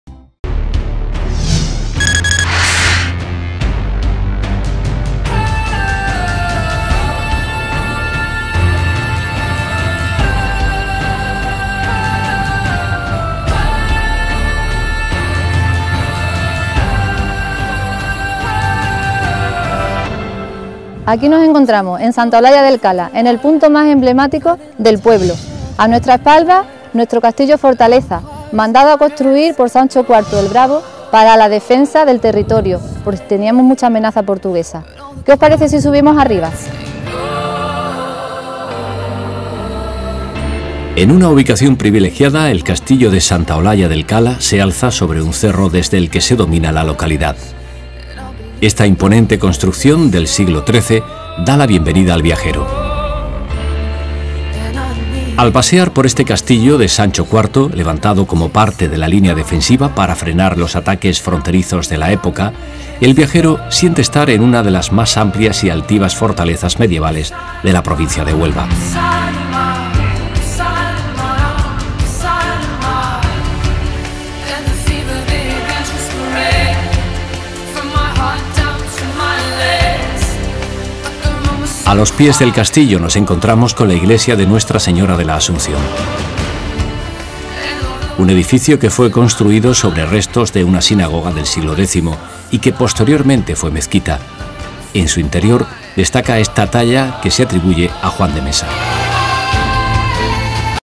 Castillo de Sancho IV e Iglesia de la Asunción. Santa Olalla del Cala. Huelva
El castillo fortaleza de Santa Olalla del Cala se alza sobre un cerro desde el que domina la localidad del mismo nombre, en la provincia de Huelva. Fue declarado Bien de Interés Cultural en 1993.

La Iglesia de Nuestra Señora de la Asunción es del siglo XIV y se conjugan los estilos gótico y múdejar. Hacia el siglo IX o X fue sinagoga, conservando de entonces sólo 2 columnas. Fue reformada en el siglo XVIII, y en el nuevo campanario conserva un símbolo parecido a una rueda y resulta estar orientado a Córdoba, tal y como también estaban orientadas todas las mezquitas del califato.